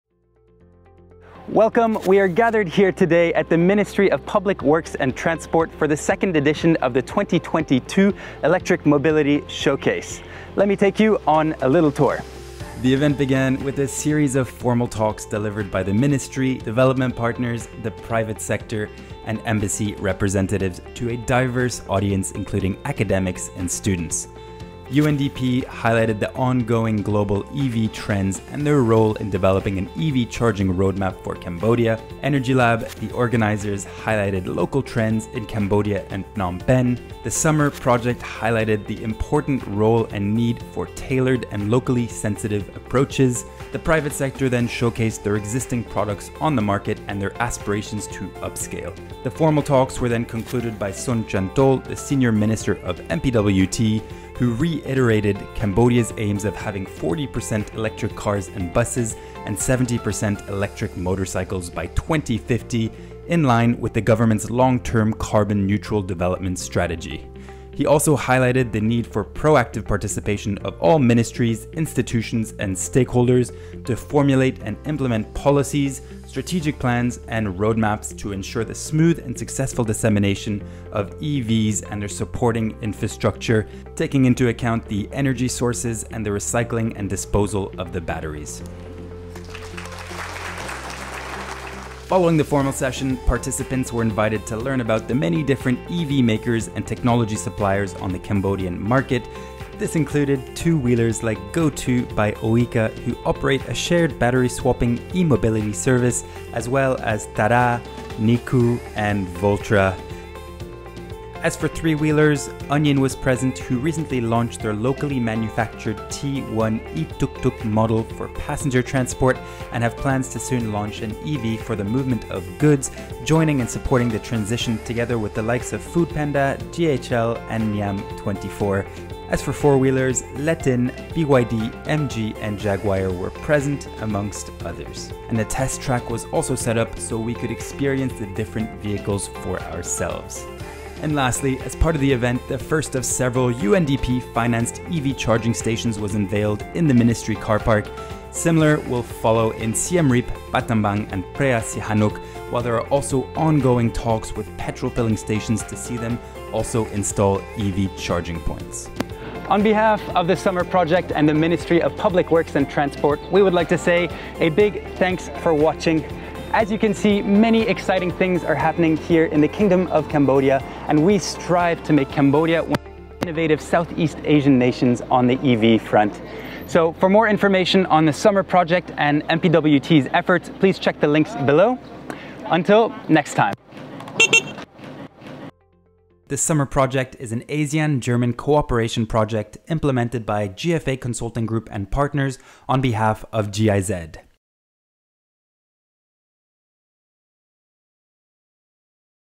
Cambodia 🇰🇭 – ELECTRIC VEHICLE SHOWCASE 2022 (MPWT)
On the 26th of March we attended the 'EnergyLab Cambodia Electric Mobility Showcase 2022' at the Ministry of Public Works and Transport (MPWT). This was an open interactive exhibition bringing together the public and private sector for a fun-filled day of learning and testing out the latest EV technology. ⚡🔌 🛵

Featuring EV technology from: Oyika, Voltra, Jaguar Land Rover, BYD, Thada, Onion, MG Motors, Miku, LETIN AUTO

SMMR is an ASEAN-German Cooperation project implemented by GFA Consulting Group GmbH and partners on behalf of Deutsche Gesellschaft für Internationale Zusammenarbeit (GIZ) GmbH.